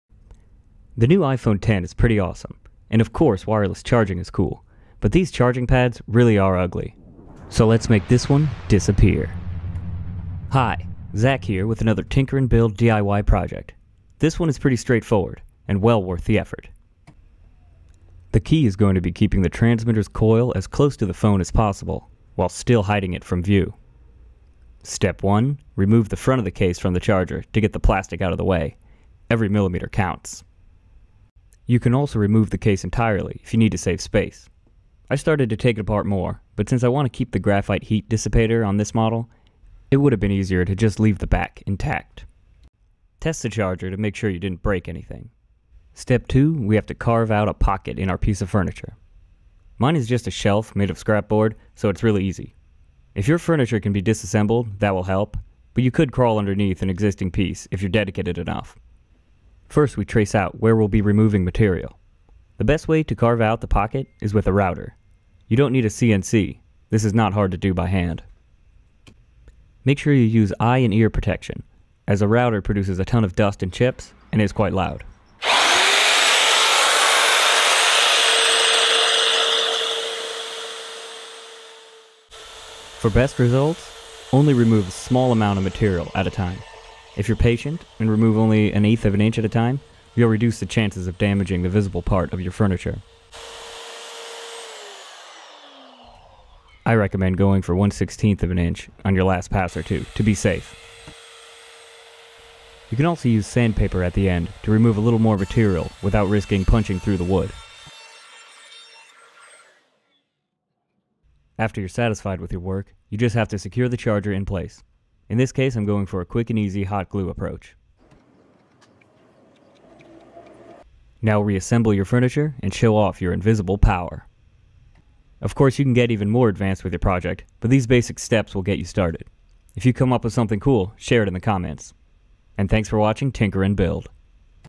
Invisible Wireless Charger
Invisible Wireless Charger DIY project.

Learn to make a hidden wireless charger for your iPhone X, 8, or compatible Android smartphone. Made with easily accessible and inexpensive parts.

[✓] Cheap wireless charger
[✓] High Speed wireless charger
[✓] Wood router